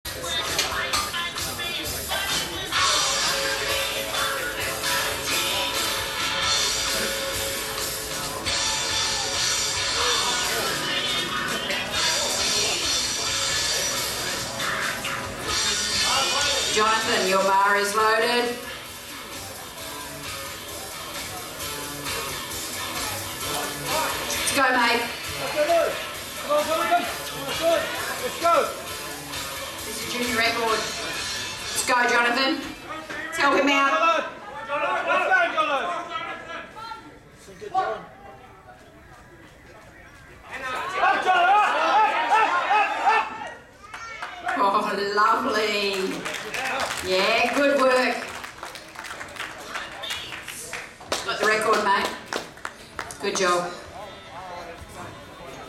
Jono Parsons - Squat 272.5kg
2012 Oceania Powerlifting Championships
Sydney, 7 December

Jono Parsons (NZL), Under 120kg Junior class
Squat: Second attempt. 272.5kg